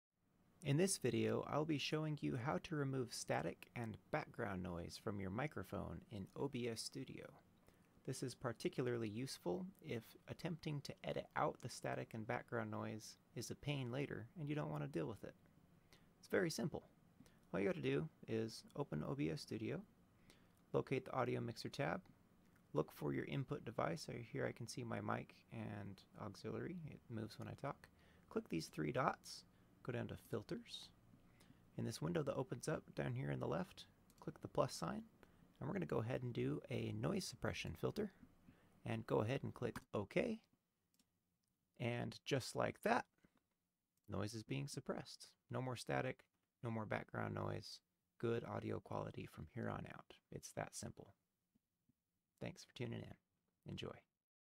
How To Remove Static and Background Noise in OBS Studio [EASY!]
In this video I show you how to remove static / background noise from your microphone or other input source in OBS Studio.